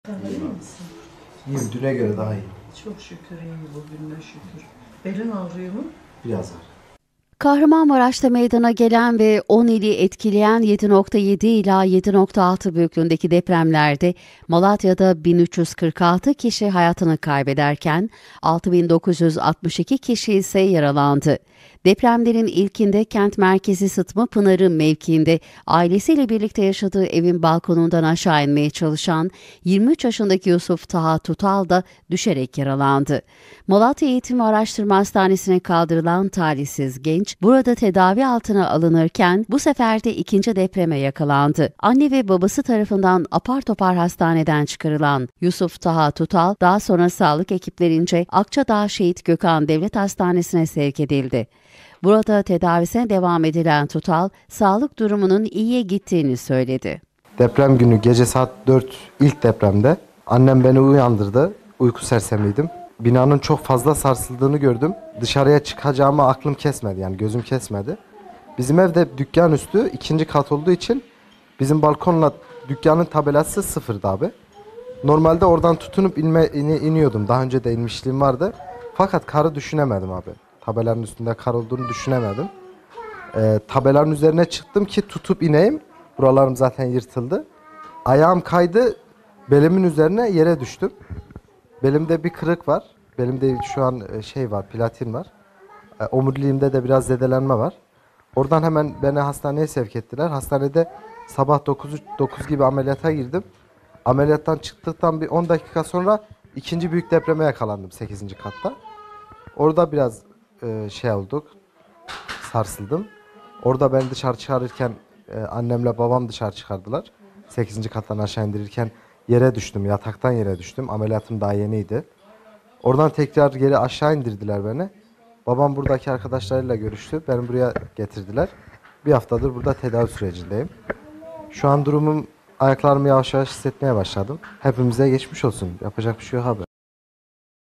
İlk Depreme Evinde İkinci Depreme Hastanede Yakalandı
Malatya’da yaşayan 23 yaşındaki Yusuf Taha Tutal Kahramanmaraş merkezli meydana gelen depremlerin ilkinde yaşadığı evin balkonundan inmeye çalışırken aşağı düşerek yaralandı. Kaldırıldığı hastanede de ikinci depreme yakalanan talihsiz genç daha sonra başka bir hastaneye sevk edildi.